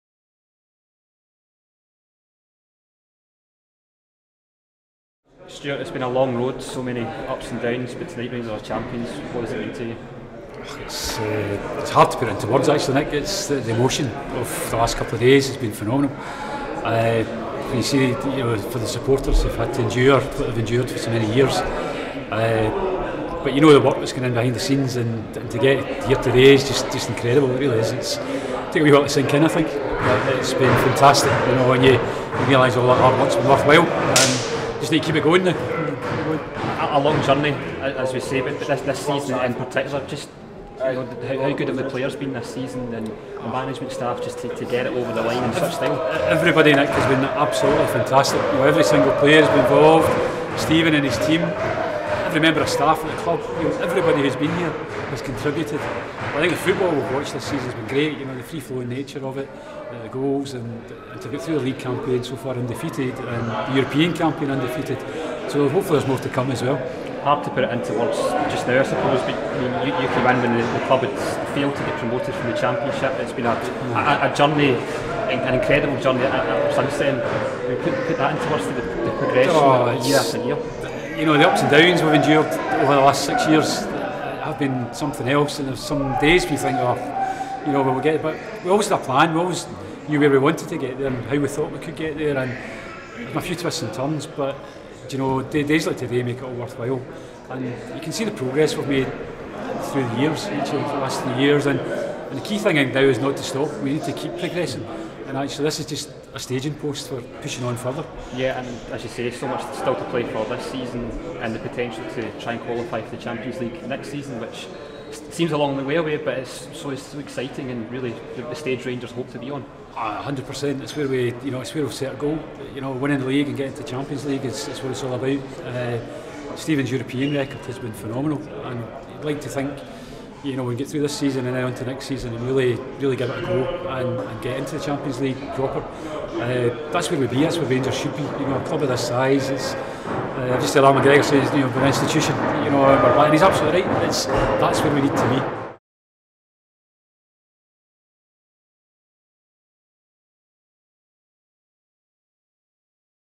Champion55 | Stewart Robertson
Managing Director Stewart Robertson speaks to RTV after Rangers clinched 55 today.